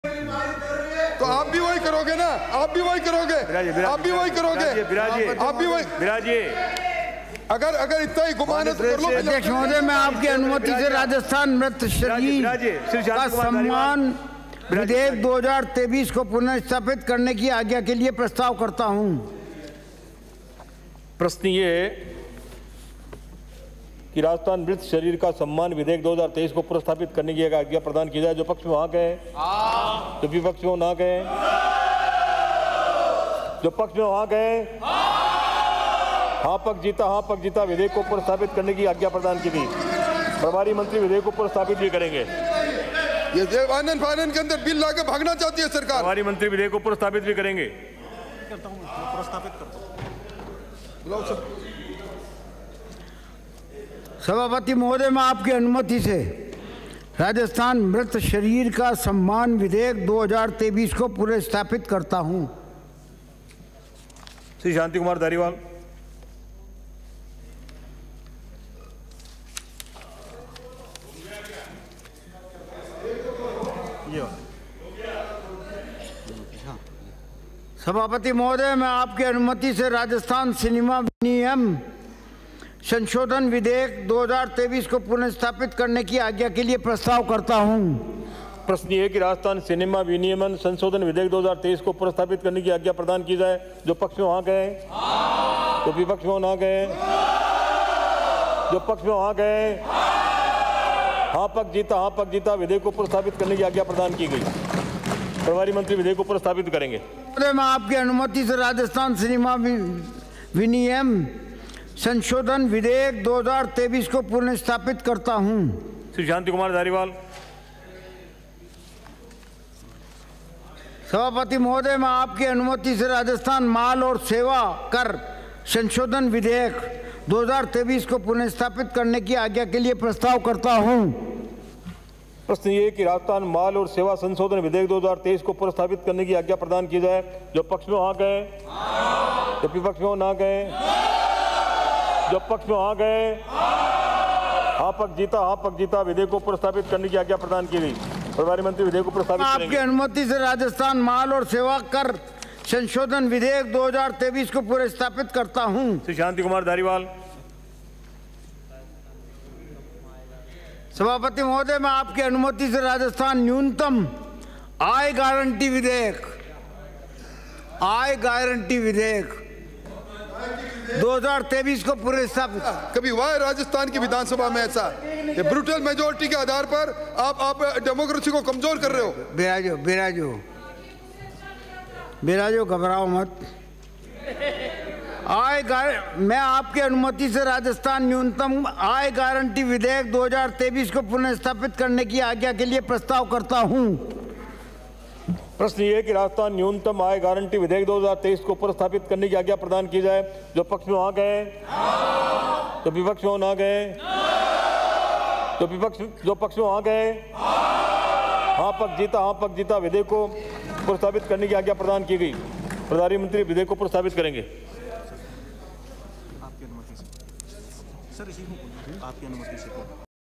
सदन में आनन फानन में आया Rajasthan मृत शरीर का सम्मान विधेयक, Shanti Dhariwal ने दिया प्रस्ताव
सदन में आनन फानन में आया राजस्थान मृत शरीर का सम्मान विधेयक पुर:स्थापित
शांति धारीवाल ने सदन में राजस्थान सिनेमा विनियम विधेयक पुर:स्थापित
राजस्थान माल और सेवा कर संशोधन विधेयक पुर:स्थापित
शांति धारीवाल ने रखा राजस्थान न्यूनतम आय गारंटी विधेयक 2023 पुर:स्थापित प्रस्ताव